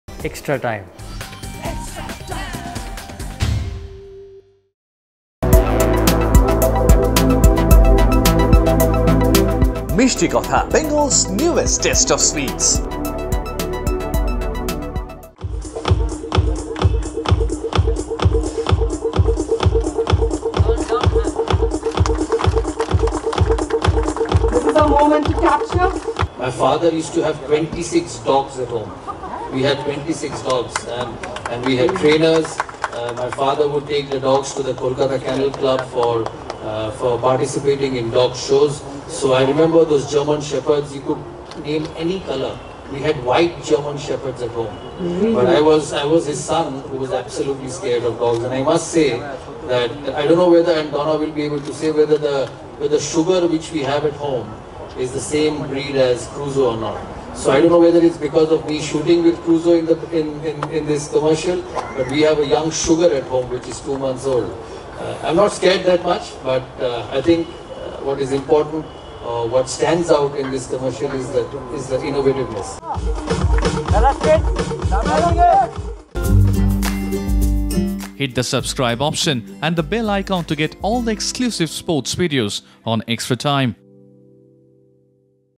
Watch: Do you know how many dogs were there at Sourav Ganguly's house?
Editor: Sandip Kar